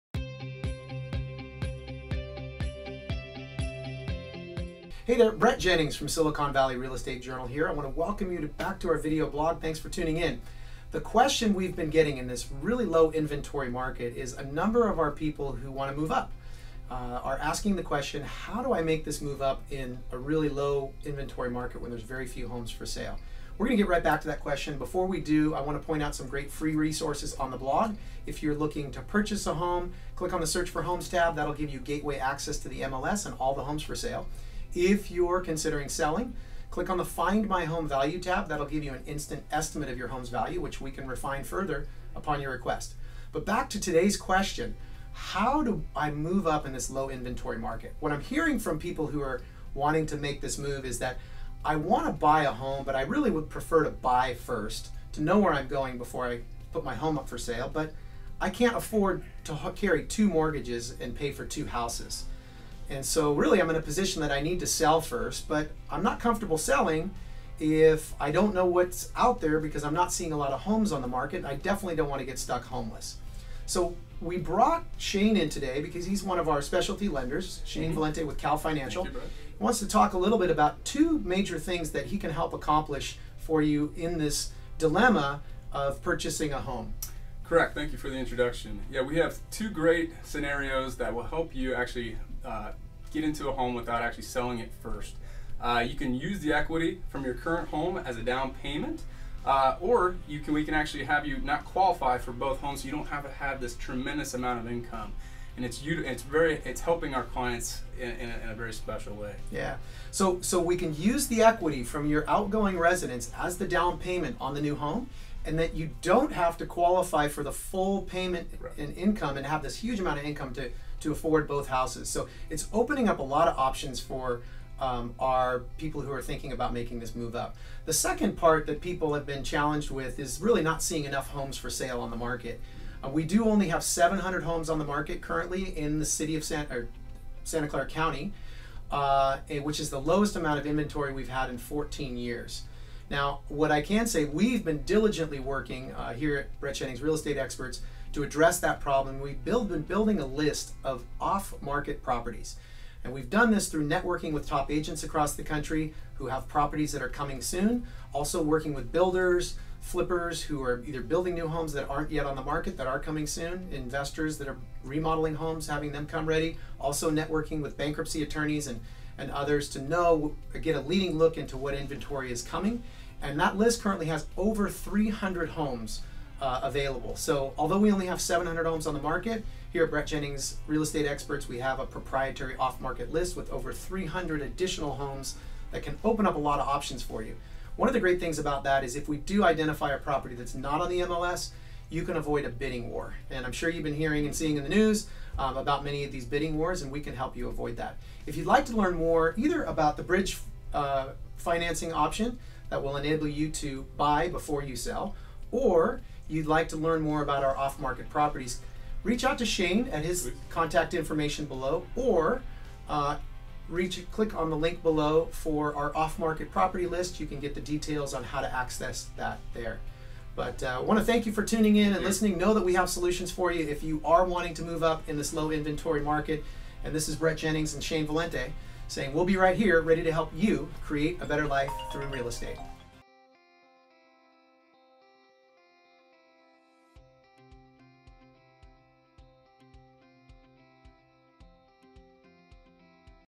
Brett Jennings Real Estate Experts: How to Move Up In a Tight Housing Market
Making a move up in our market can be a little tricky. Many home sellers are hesitant to list their homes because of the lack of selection available for them when it comes time to buy. Our friend Shane Valente of Kal Financial joined us recently to talk about a few different options for homeowners in this position. One popular solution is bridge financing, which will allow you to buy a home before you sell with the equity you already have. To learn more, watch this short video.

Selling Your Silicon Valley Home? See Your Home Value Instantly

Buying a Silicon Valley Home? Search All Homes on MLS

Brett Jennings
eXp Realty - Brett Jennings Real Estate Experts